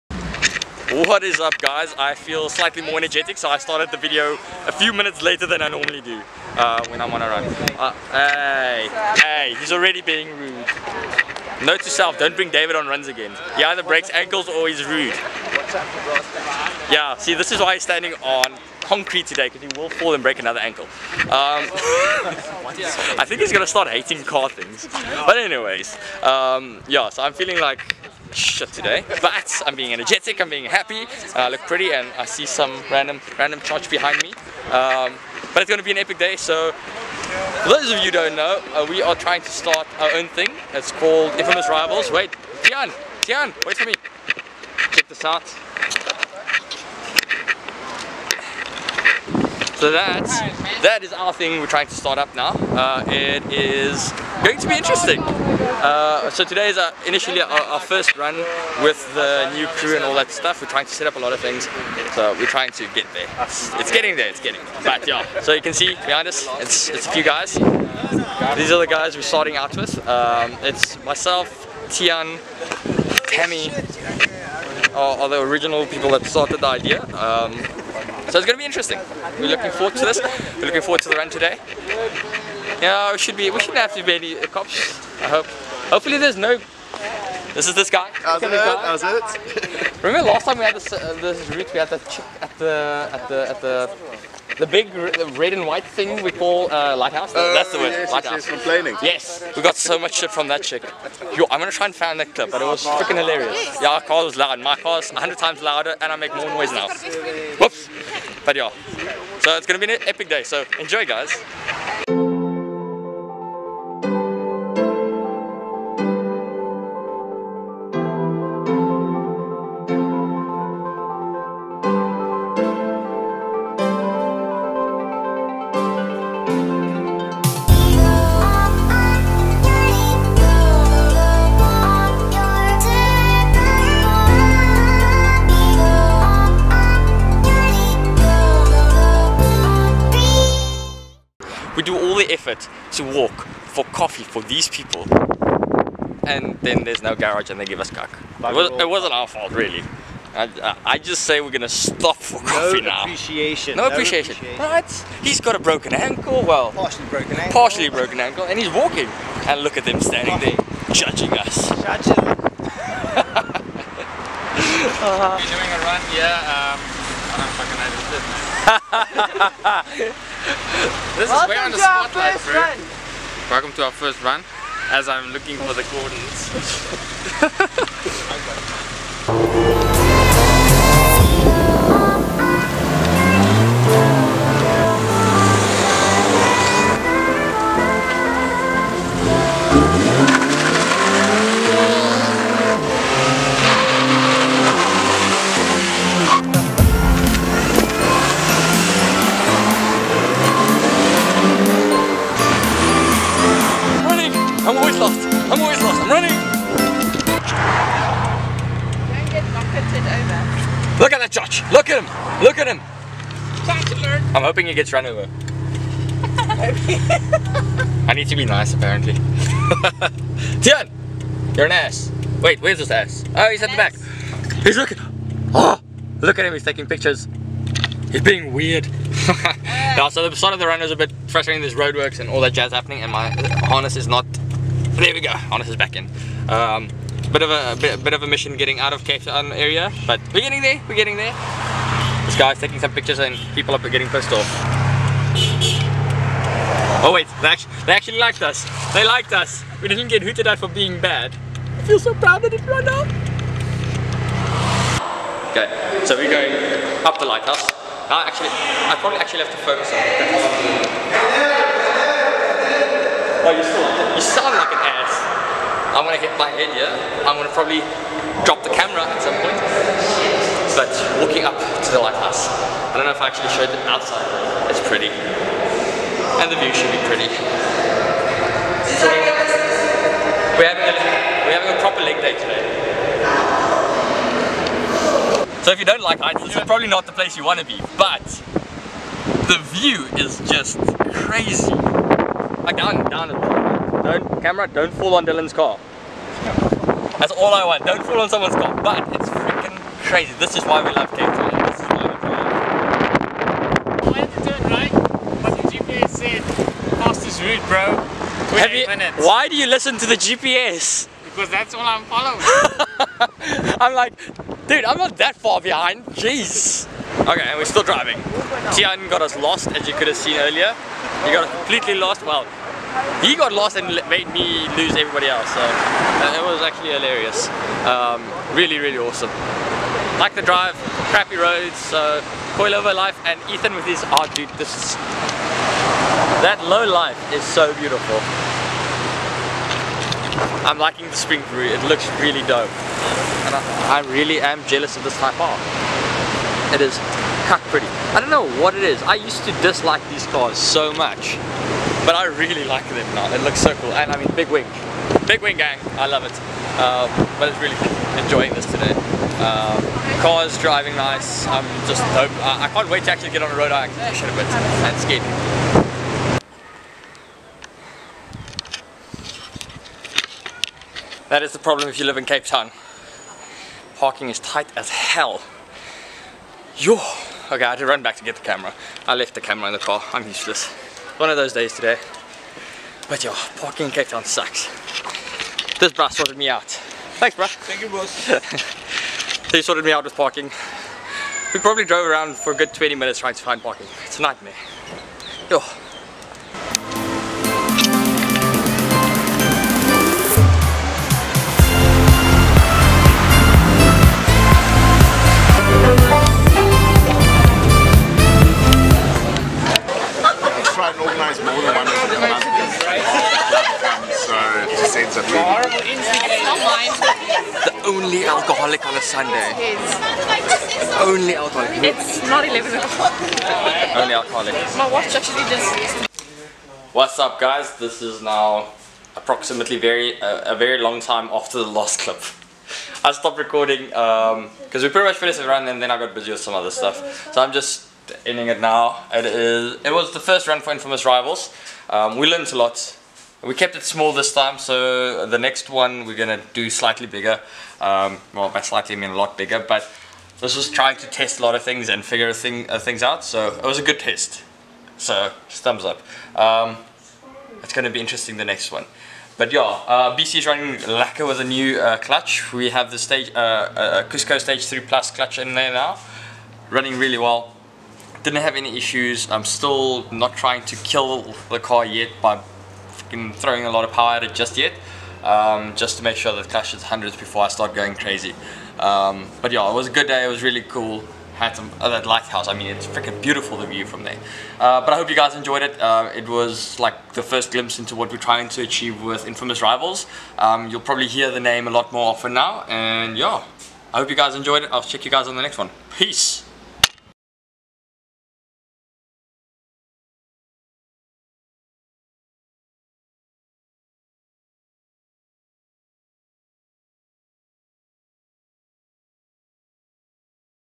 Rise of Infamous Rivals | Just a good Sunday | Nemesis Vlogs
So finally after a lot of background planning, designing etc we had our first run under Infamous Rivals! So there are a lot of plans in the pipeline and might take a while to set things up, but it is going to be epic! We are going to be doing things more often in and around Cape Town for now, but there will be a lot more information coming in the future, for now we are still trying to find our feet. This was the first run of 2018 and we learnt quite a few things, so we can just improve from here!

O M II N

Free Background Music can be used by any YouTube user including their monetised content.